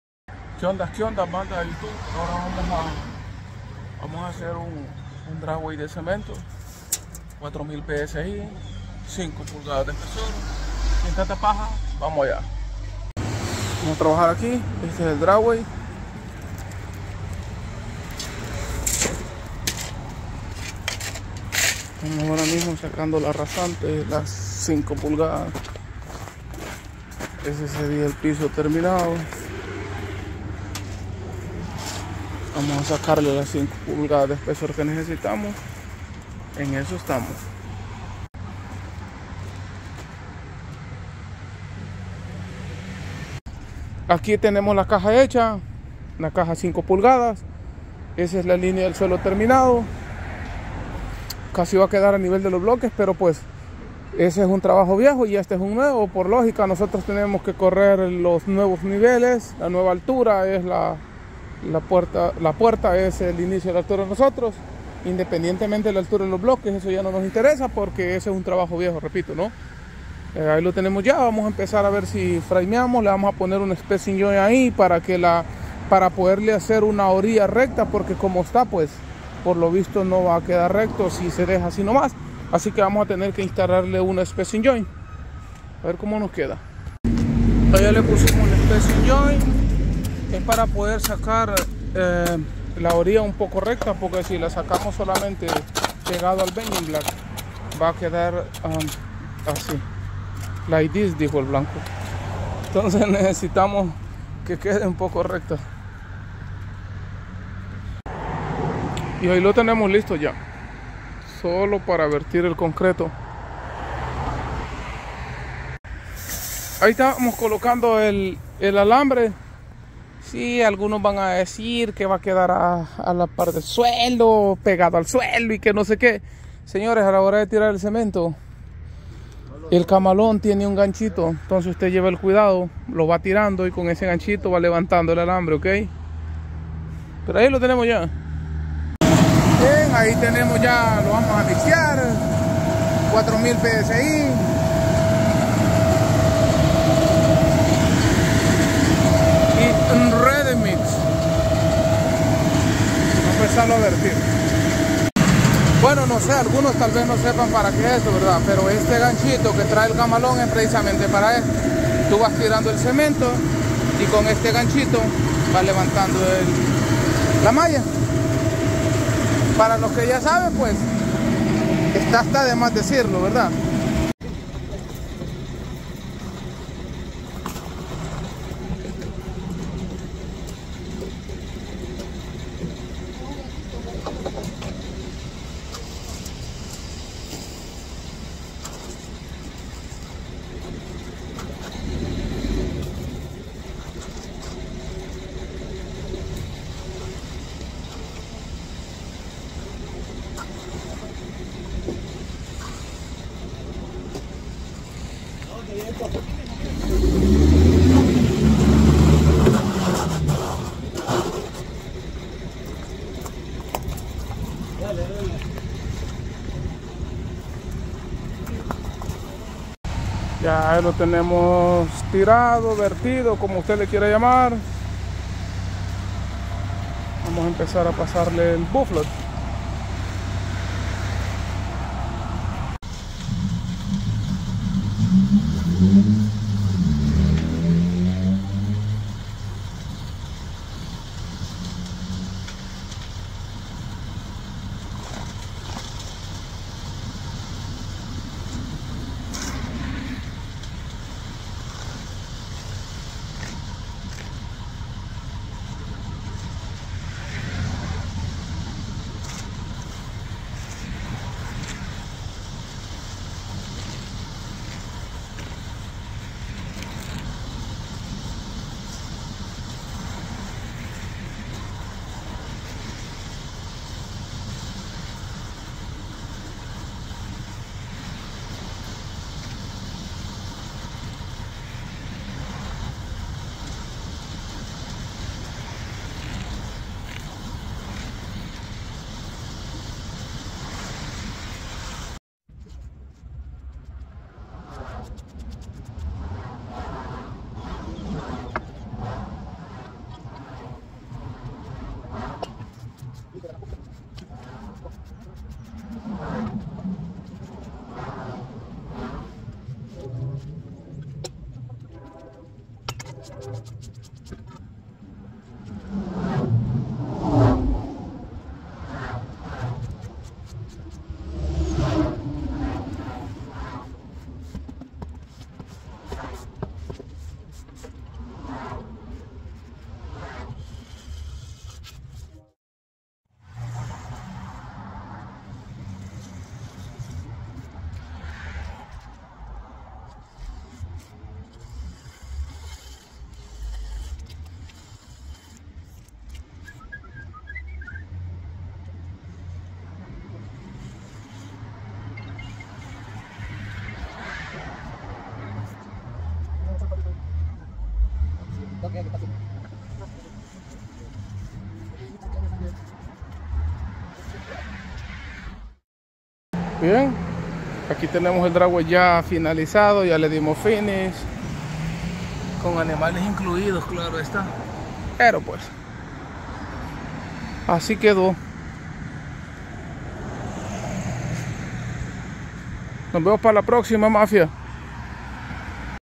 como hacer un driveway de concreto año 2,023
una maneera de como hacimos un driveway de concreto de forma sensilla solo 3 personas virtiendo 6 yardas de cemento...